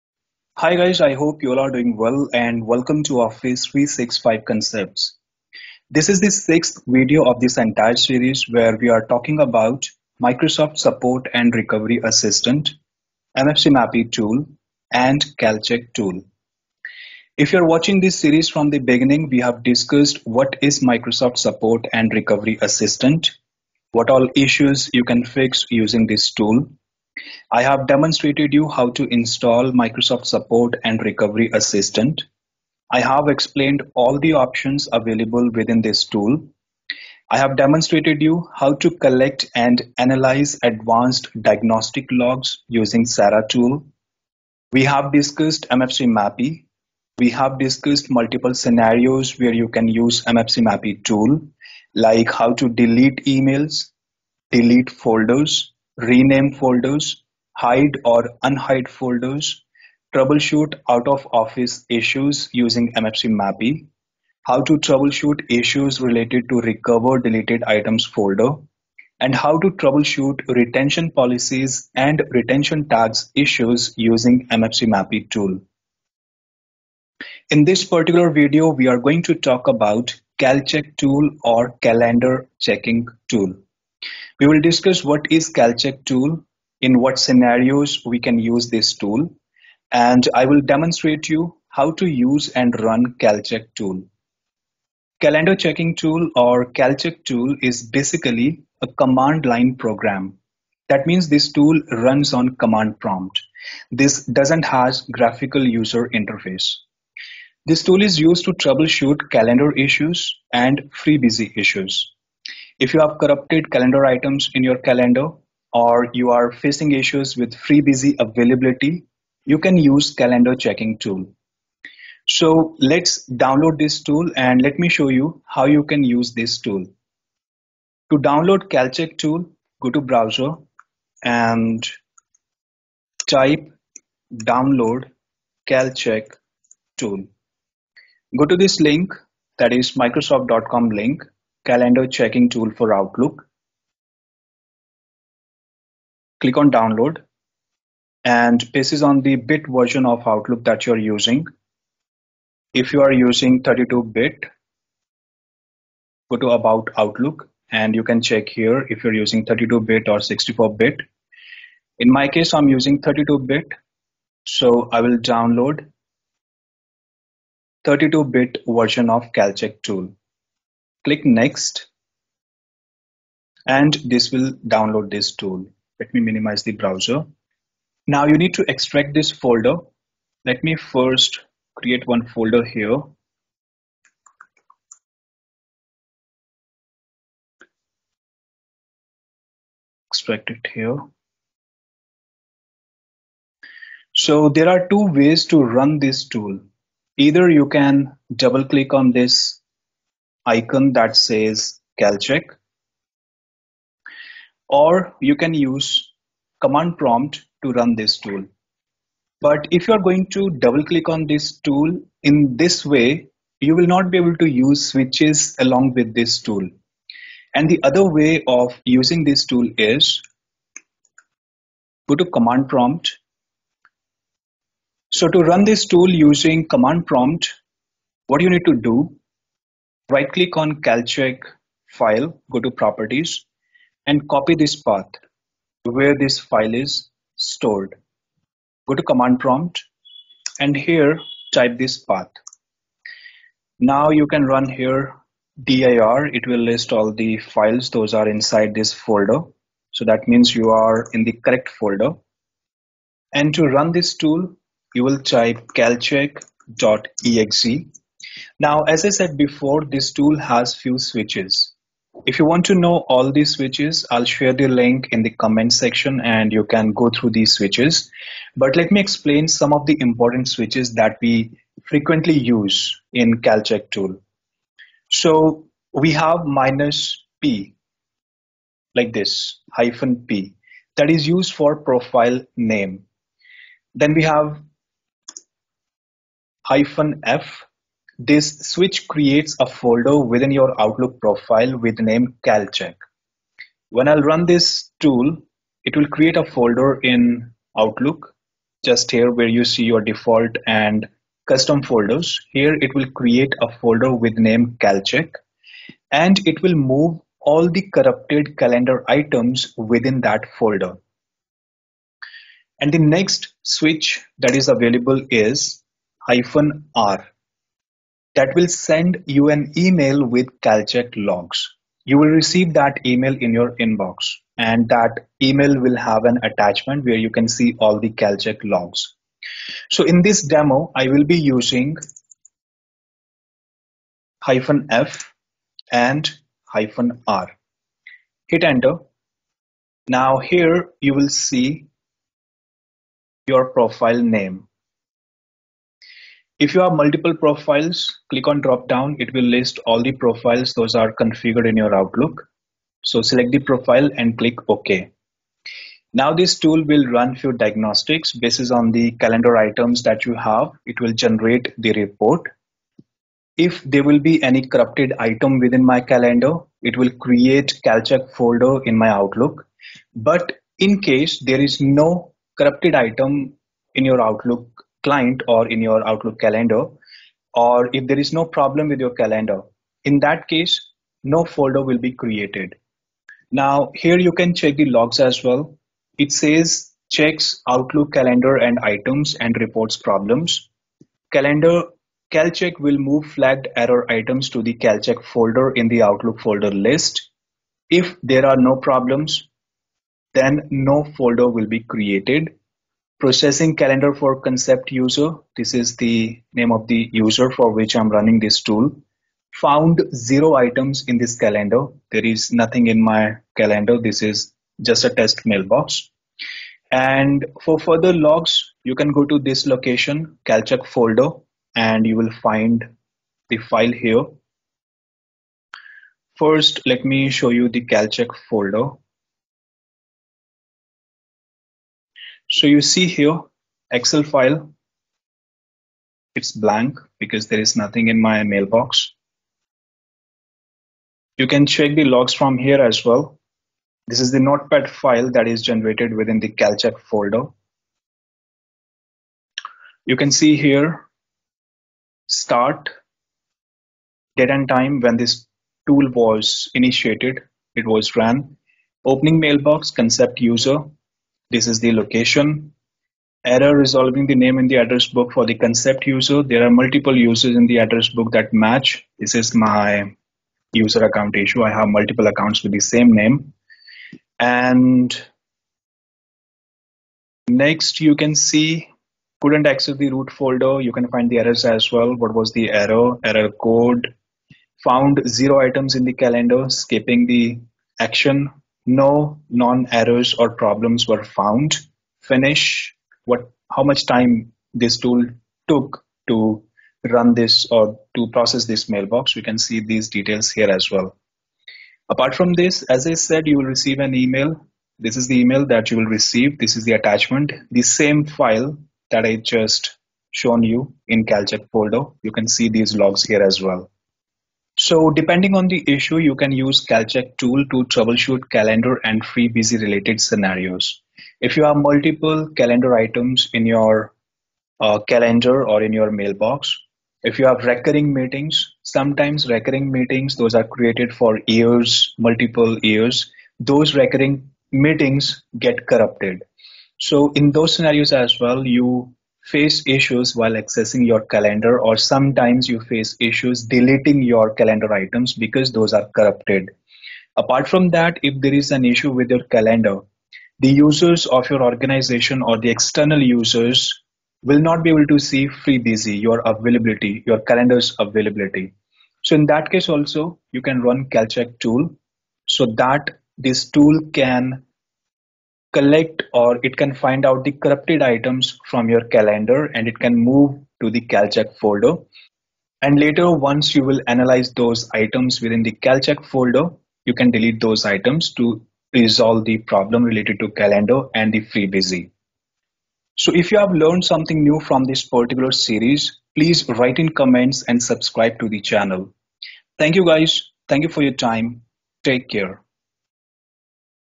CalCheck Tool | Calendar Checking Tool | How to use CalCheck tool
This is the 6th video of the series where we are discussing 3 important tools when it comes to troubleshoot Exchange Server or Exchange Online environment issues. Microsoft Support and Recover Assistant (SaRA), MFCMAPI, CalCheck Tool (Calendar Checking Tool).

- Hit the THANKS button in video!